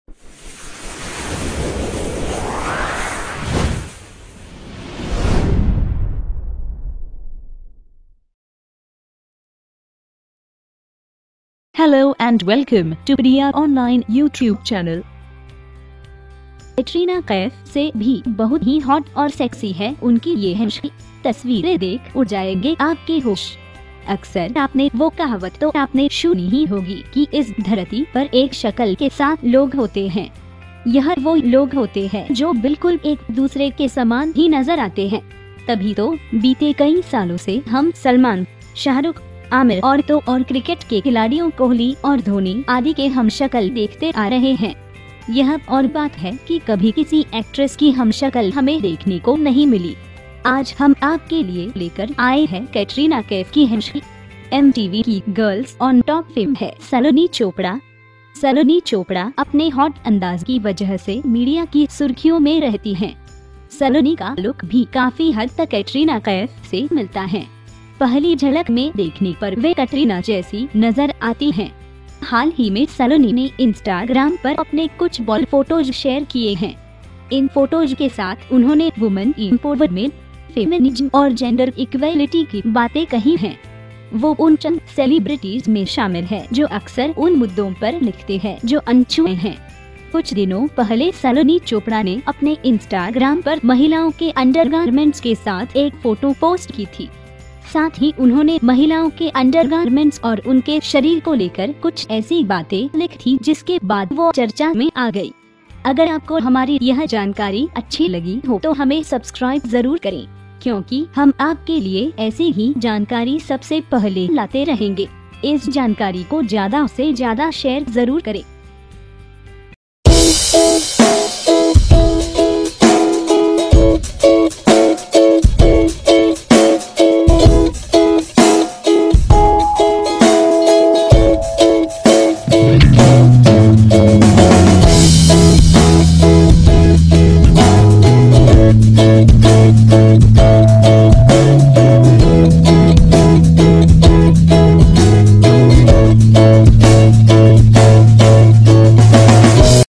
कैटरीना कैफ से भी बहुत ही हॉट और सेक्सी है उनकी ये हमशक्ल, यकीन न आये तो खुद देखो
आज हम आपके लिए लेकर आए हैं कैटरीना कैफ की हमशक्ल। MTV की गर्ल्स ऑन टॉप फेम हैं सलोनी चोपड़ा। सलोनी चोपड़ा अपने हॉट अंदाज की वजह से मीडिया की सुर्खियों में रहती हैं।

सलोनी का लुक भी काफी हद तक कैटरीना कैफ से मिलता है। पहली झलक में देखने पर वे कटरीना जैसी नज़र आती हैं।

हाल ही में सलोनी ने इंस्टाग्राम पर अपने कुछ बोल्ड फोटोज शेयर किए हैं। इन फोटोज के साथ उन्होंने वुमन इम्पॉवरमेंट, फेमिनिज्म और जेंडर इक्वैलिटी की बातें कही हैं।